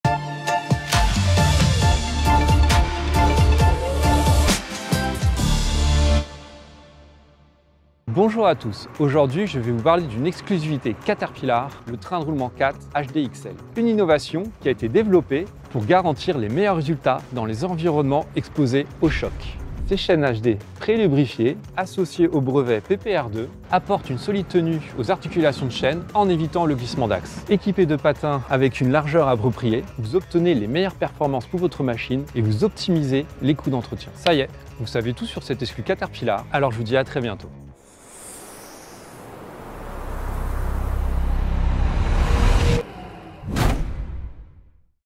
BERGERAT MONNOYEUR - Paroles d'Experts #01 TDR CAT HDCL D5 & D6 (2639761-01)
Concessionnaire officiel des engins de travaux publics Caterpillar, Bergerat Monnoyeur nous confie la réalisation d'une série de capsules vidéo.

Objectif : dans un format court et accrocheur, valoriser leur gamme de produits, notamment les trains de roulements (chenilles), en donnant la parole aux experts de la marque.

Dans cette première vidéo, Rudy présente le train de roulement TDR CAT HDXL garantissant les meilleurs résultats dans les environnements exposés aux chocs.

Année de production : 2022
Thème, format : innovation, engins de chantiers, capsules vidéo